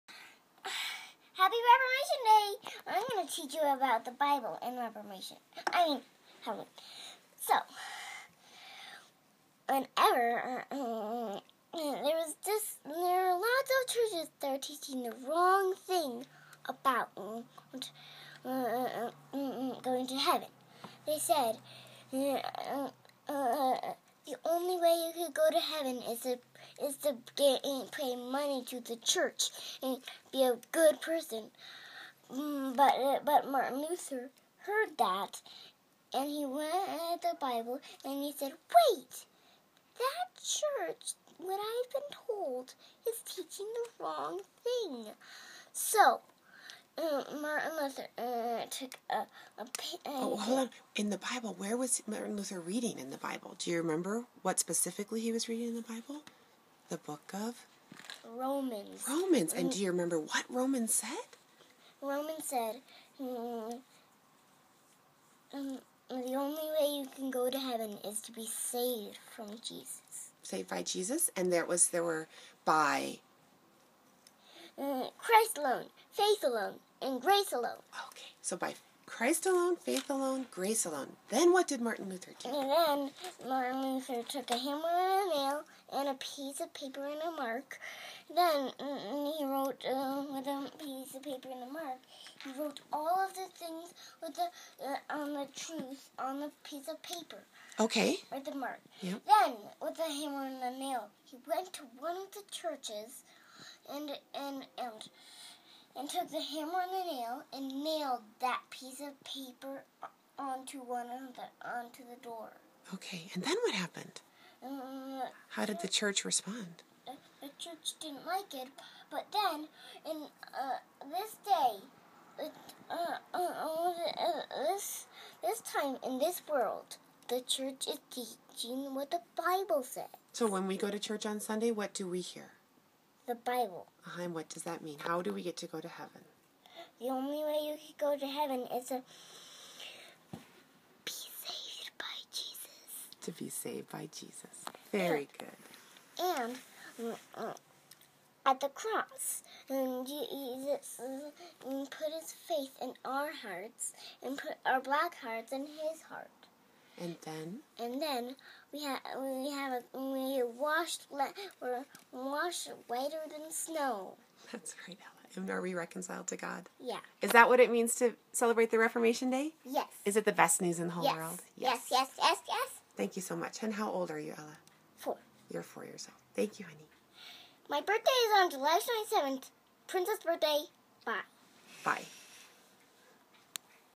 A Four Year Old Explains Reformation Day
Our four year-old is very excited that today is Reformation Day and she wants to tell us all about it.